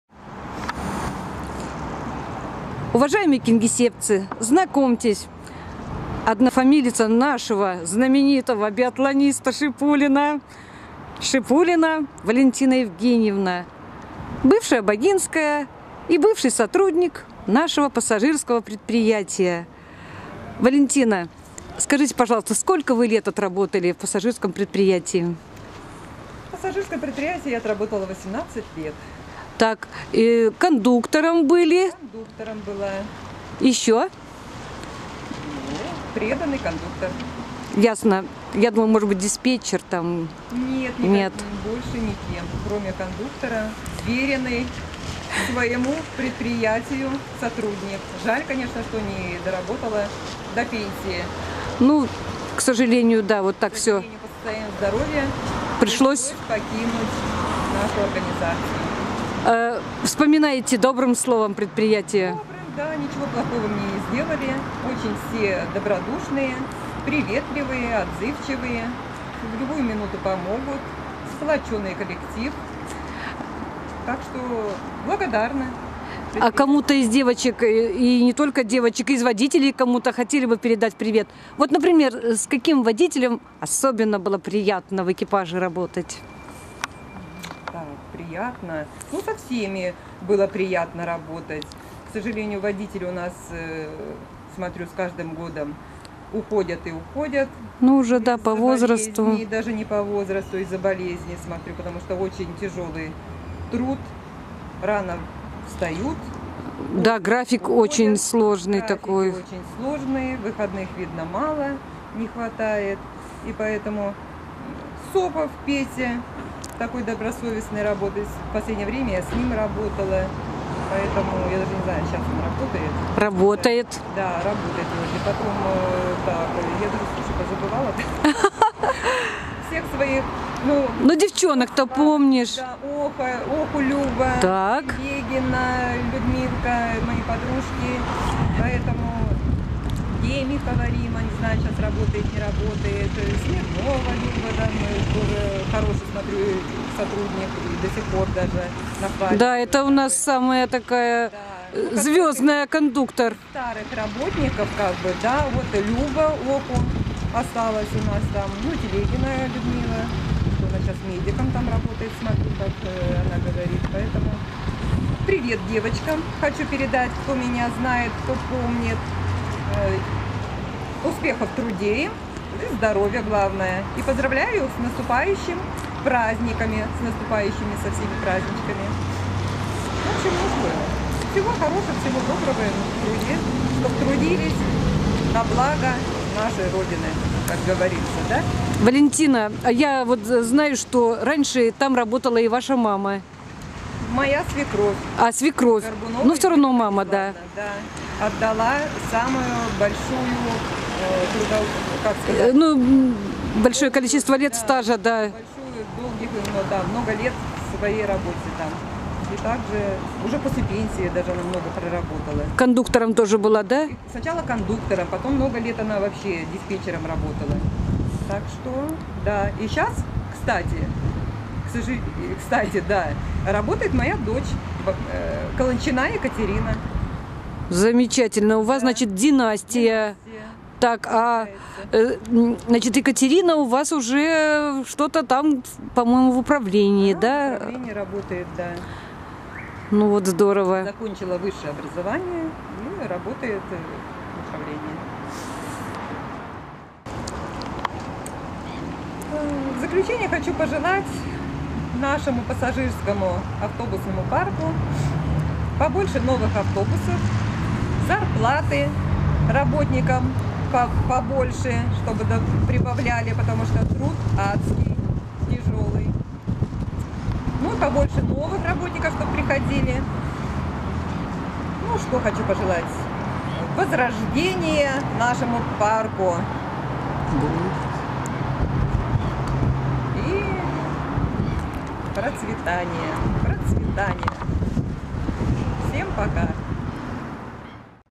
Шипулина Валентина Евгеньевна из АО "Кингисеппский автобусный парк"
Друзья, знакомим вас с еще одной семейной династией нашего пассажирского предприятия АО "Кингисеппский автобусный парк". Кондуктора Шипулину Валентину Евгеньевну помнят многие кингисеппцы. Красивая статная блондинка с приветливой улыбкой, уважительная и внимательная к пассажирам ,всегда была украшением автобусного парка.

18 лет трудового стажа отдала Валентина этому предприятию и вспоминает о том времени с улыбкой и по - доброму, передает привет всем, кто ее знает и помнит, а также поздравляет всех с наступившими майскими праздниками.

Как рассказала нам Валентина Евгеньевна, на нашем автобусном предприятии долгие годы трудилась ее свекровь, Горбунова Ядвига Брониславовна, которая начинала работать кондуктором, а позже стала диспетчером.

Что очень приятно, сейчас здесь же трудится дочь Валентины Шипулиной, Колончина Екатерина Ивановна. Девушка получила высшее образование и является бухгалтером АО "Кингисеппский автобусный парк". Как здорово, что на наших предприятиях живут традиции, нельзя потерять это ценное зерно, необходимо привлекать на работу молодежь